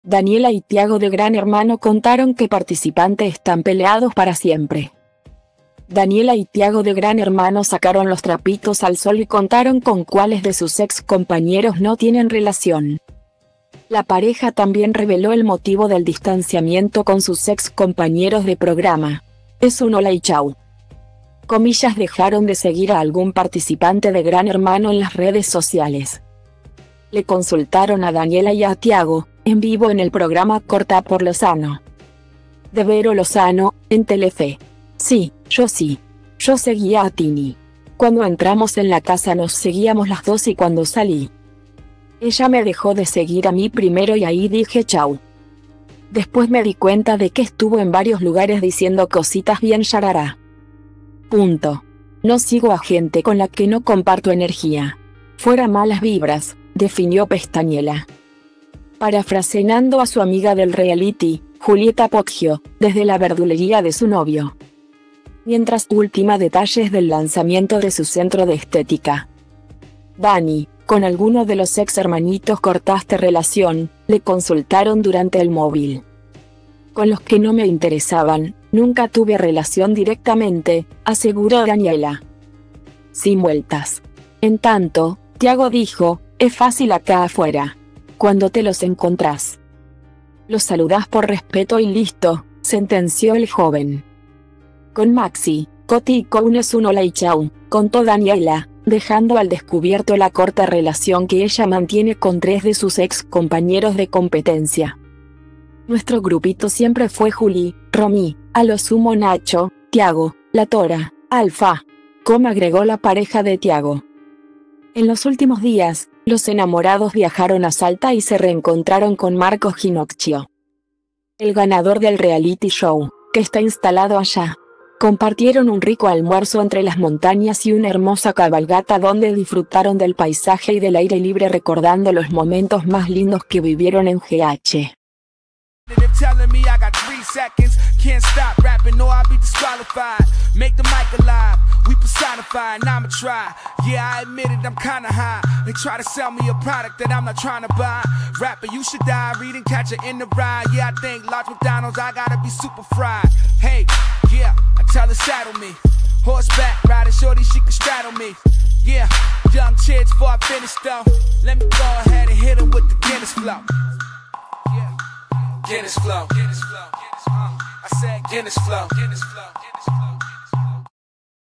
Daniela y Thiago de Gran Hermano contaron qué participante están peleados para siempre
Daniela y Thiago de Gran Hermano sacaron los "trapitos al sol" y contaron con cuales de sus ex compañeros no tienen relación.la pareja también reveló el motivo del distanciamiento con sus ex compañeros de programa. "Es un hola y chau".